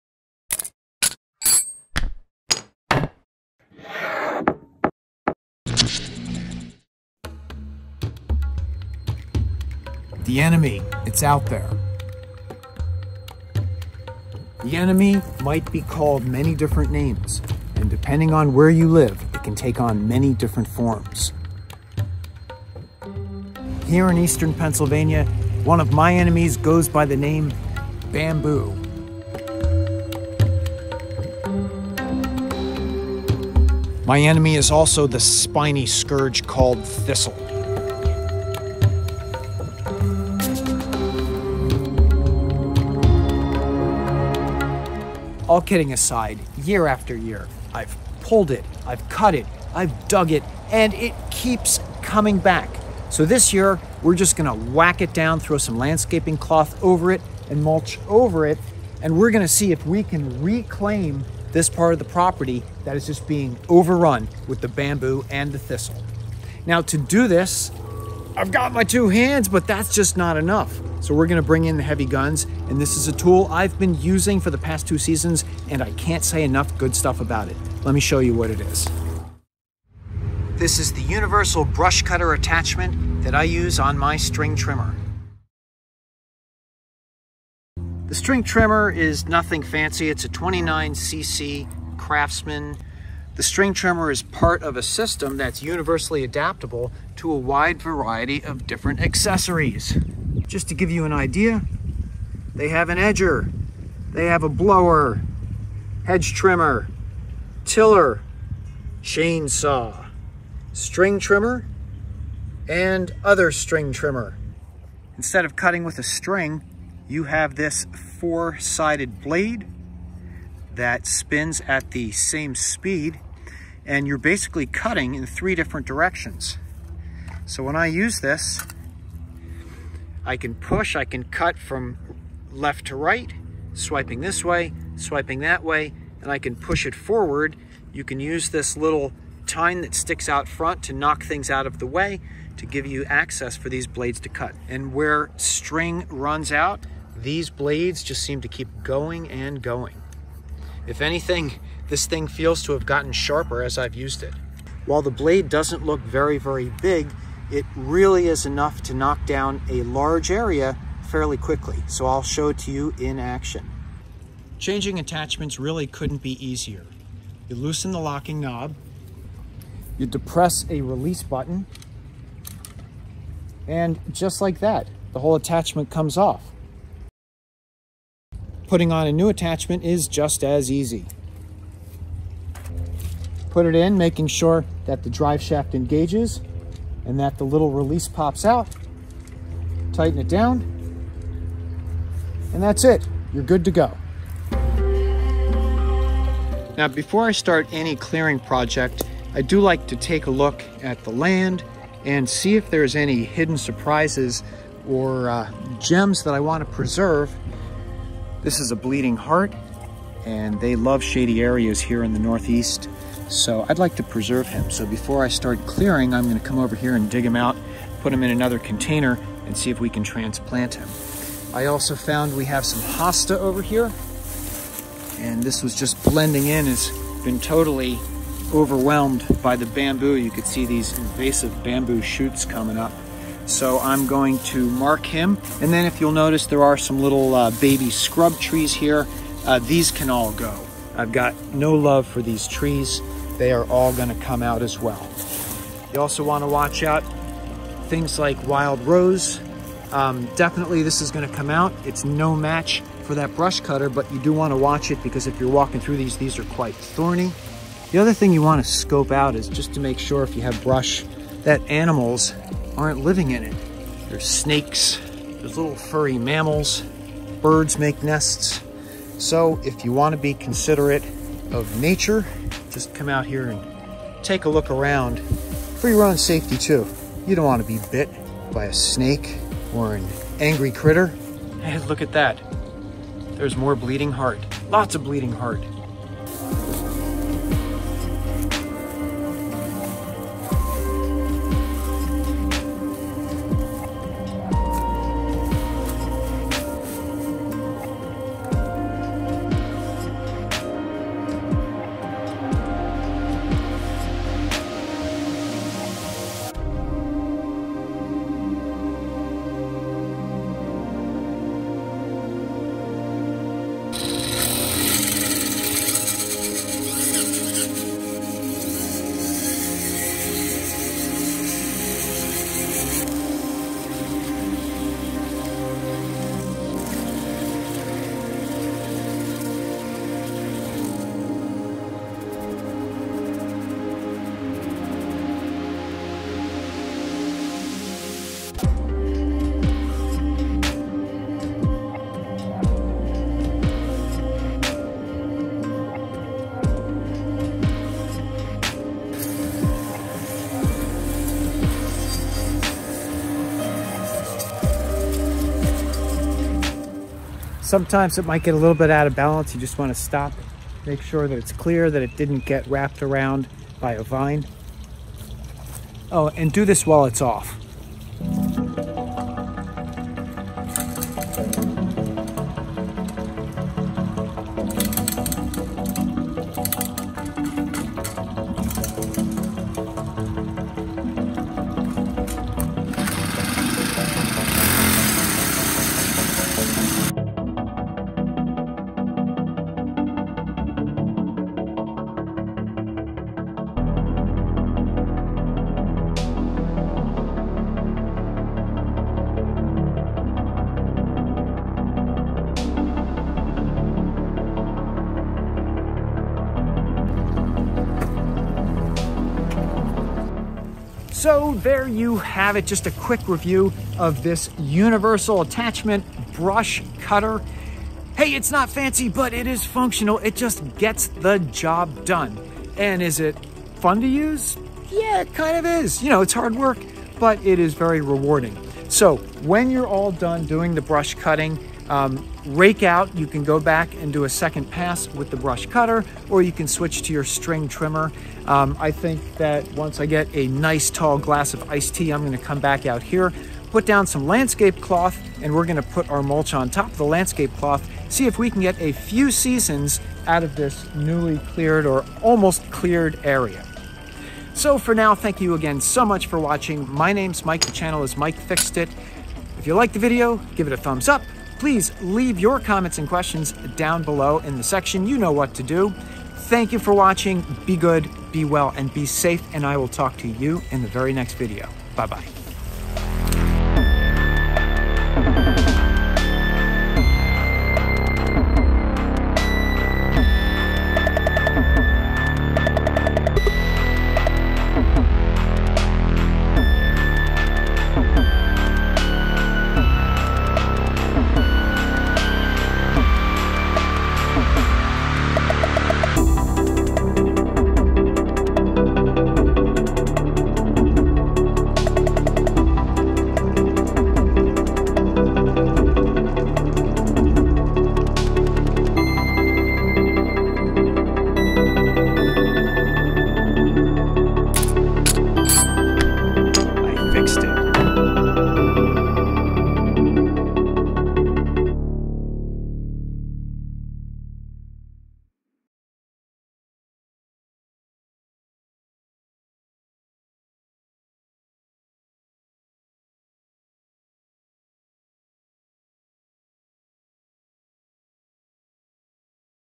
Awesome Yard Tool! | Brush Cutter String Trimmer Attachment
A ‘Must-have’ yard tool for all DIY homeowners with property! This string trimmer brush cutter attachment makes FAST work of clearing weeds, small trees, bamboo and lots more. Perfect for landscaping and taming new properties. I love this tool!

This Brush Cutter string trimmer attachment fits string trimmers that accept universal attachments. Mine is branded Craftsman 71-79244, but is essentially the same as this model if you’re interested!

Fits universal attachment trimmers from Troy-Bilt, Remington, Craftsman, Yard Machines, Yardman, Snapper, Bolens, Murray, Ryobi, Greenworks, Kobalt and other major brands.